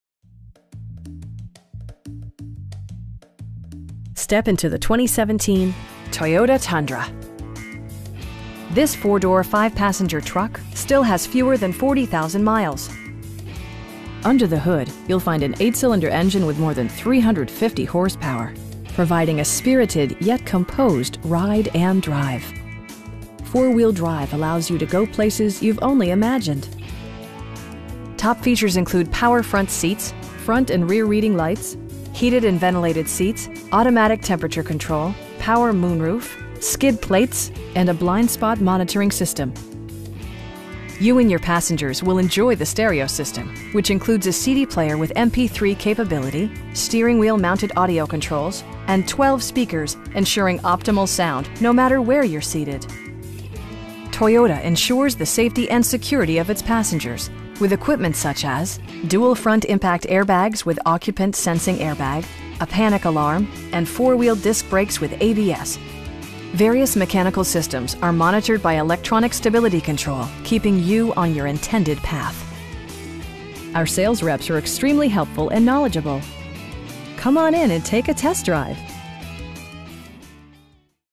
2017 Toyota Tundra 4WD Platinum in Charlotte, NC 28269
Mercedes-Benz of Northlake
10725 Northlake Auto Plaza Blvd

Step into the 2017 Toyota Tundra. This 4 door, 5 passenger truck still has fewer than 40,000 miles! Under the hood you'll find an 8 cylinder engine with more than 350 horsepower, providing a spirited, yet composed ride and drive. Four wheel drive allows you to go places you've only imagined. Top features include power front seats, front and rear reading lights, a trip computer, heated and ventilated seats, automatic temperature control, power moon roof, skid plates, and a blind spot monitoring system. You and your passengers will enjoy the stereo system, which includes a CD player with MP3 capability, steering wheel mounted audio controls, and 12 speakers, providing excellent sound throughout the cabin. Toyota ensures the safety and security of its passengers with equipment such as: dual front impact airbags with occupant sensing airbag, front side impact airbags, traction control, brake assist, a panic alarm, and 4 wheel disc brakes with ABS. Various mechanical systems are monitored by electronic stability control, keeping you on your intended path. Our sales reps are extremely helpful & knowledgeable. We'd be happy to answer any questions that you may have. Come on in and take a test drive!